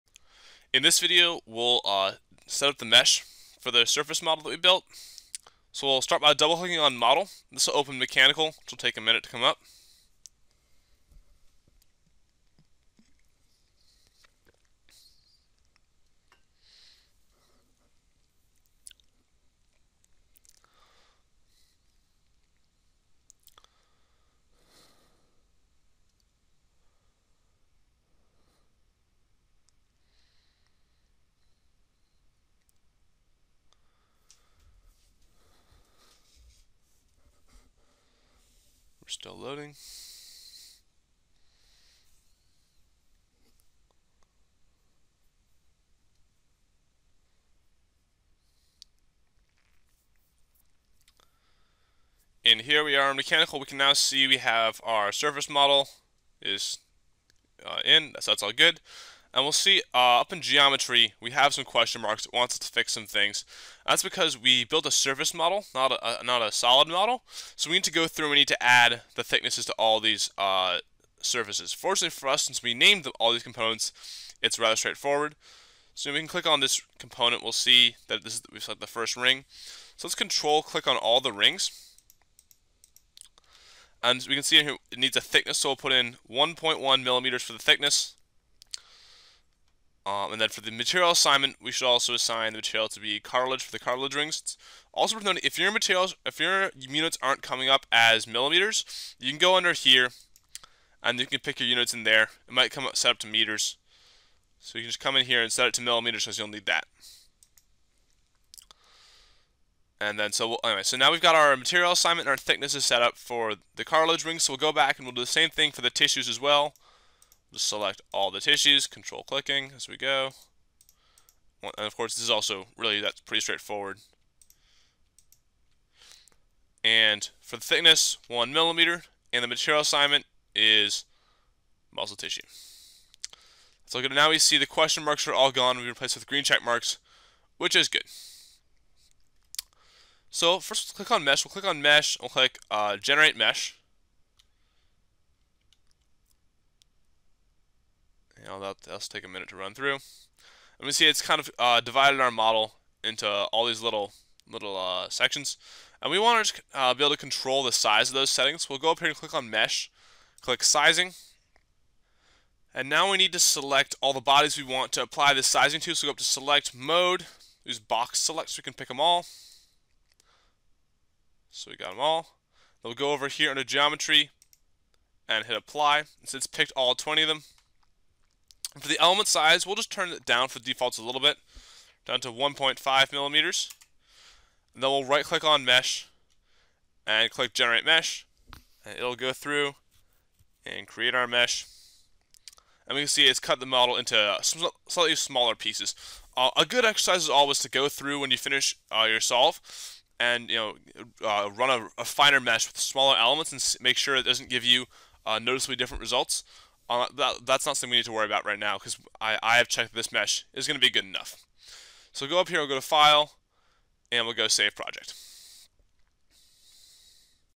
Structural Analysis of a Trachea Using Ansys Mechanical — Mesh
This video shows how to generate the mesh for the model. This video is part of the Ansys Innovation Course: Structural Analysis of a Trachea Using Ansys Mechanical. Cornell University also offers a Fluid Dynamics Simulations Using Ansys online certificate authored by Dr. Rajesh Bhaskaran.